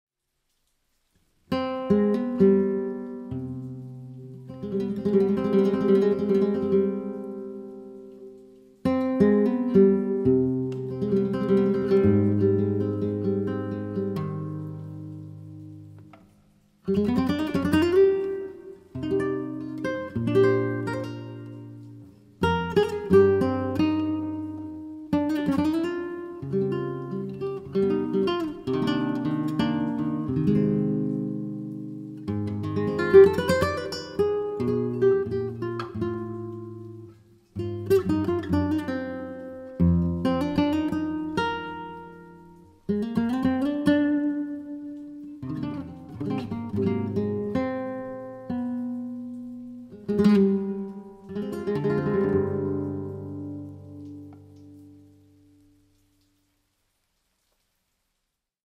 Flamenco Guitar / taranta / Ceyhun Gunes
Yağmur damlaları ve gitar... Yeni bestemden küçük bir bölüm. ☺
Rain drops and the guitar... A small part from my new comp.☺
Las gotas de lluvia y la guitarra... Un pequeño parte de mi nueva composicion.☺